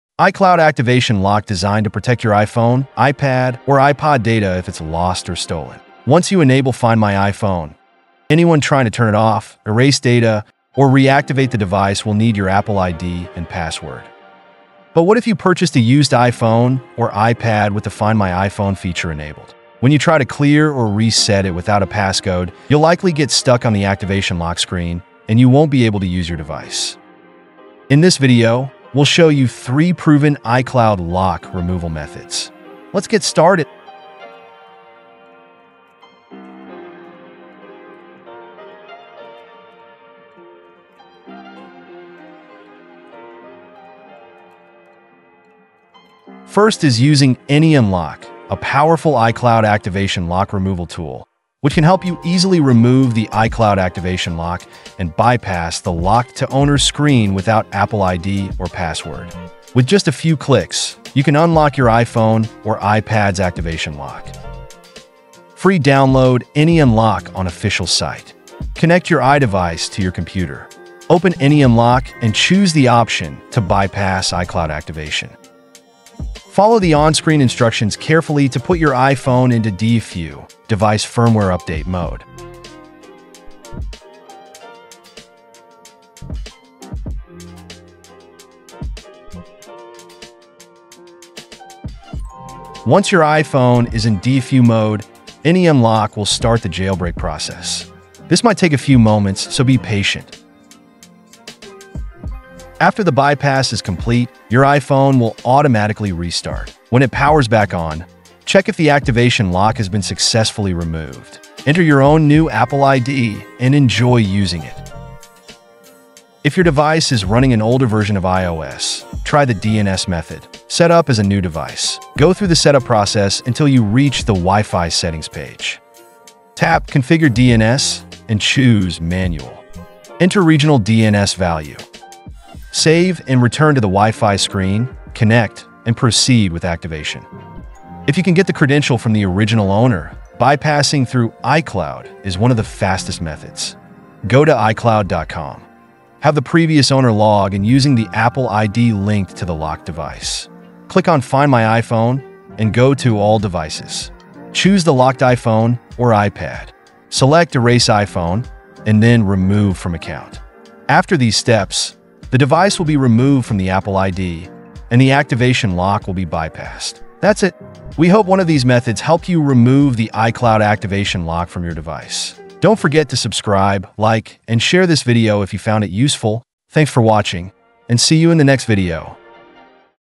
iCloud Locked? iCloud Activation Lock Removal without Password✅ | 2025
iCloud Activation Lock designed to protect your iPhone, iPad, or iPod data if it’s lost or stolen. But what if you purchased a used iPhone or iPad with the Find My iPhone feature enabled? When you try to clear or reset it without a passcode, you'll likely get stuck on the Activation Lock screen and you won't be able to use your device.

In this video, we'll show you three proven iCloud lock removal methods. Keep Watching!
00:00 Intro
00:43 Method 1: iCloud Activation Lock Removal Without Password
01:54 Method 2: DNS Method (For Older iOS Versions)
02:18 Method 3: iCloud Lock Removal through iCloud
How to Bypass Screen Time Passcode on iPad without Apple ID | No System Restore or Lost Data